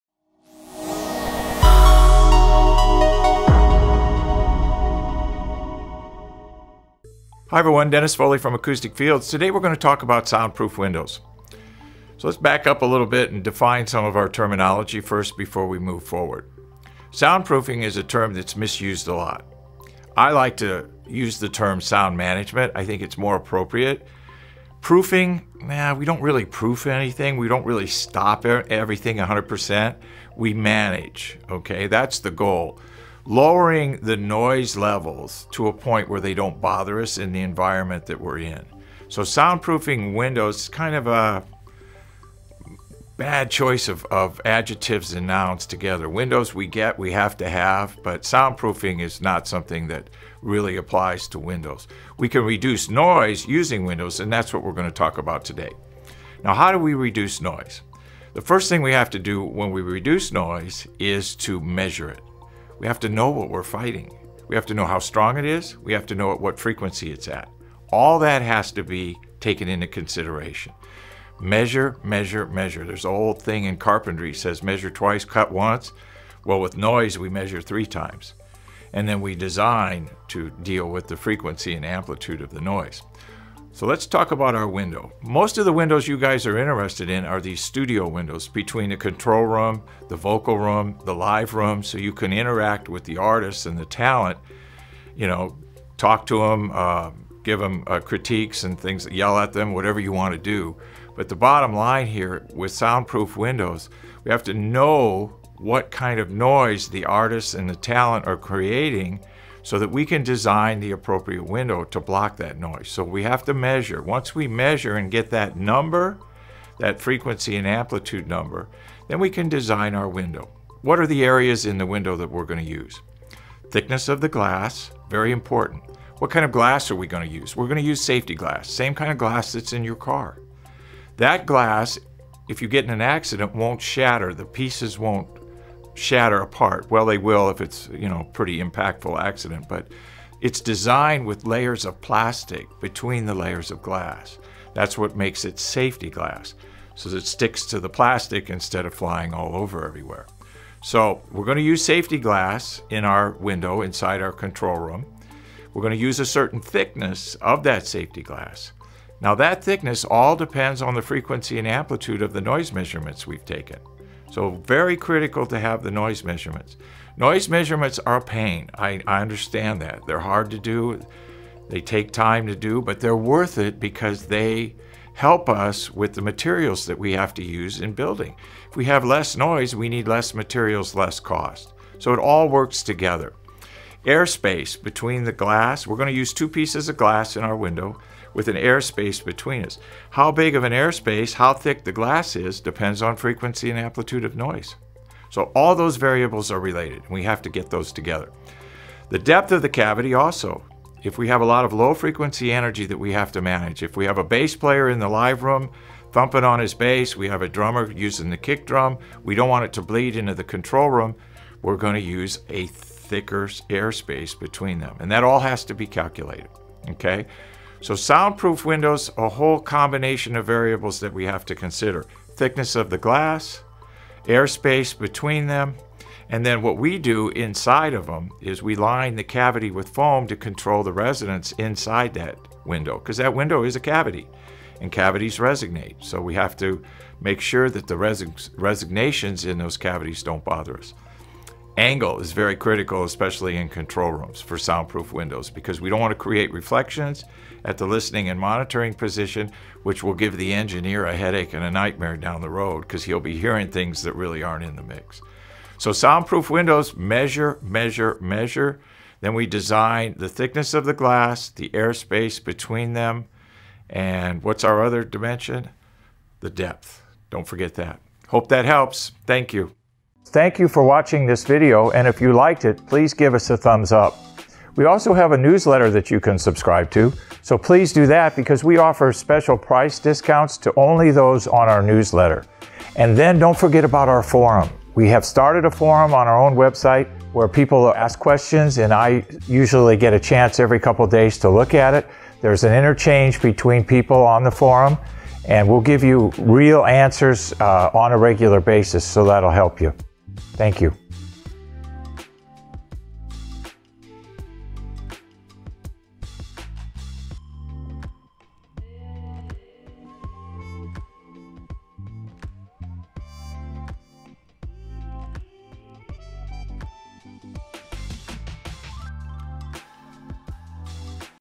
Soundproof Windows
Soundproof Windows: Is that even possible? In this video, chief acoustics engineer Dennis Foley talks about soundproofing windows and what variables we have to look at. Watch the video to find out more.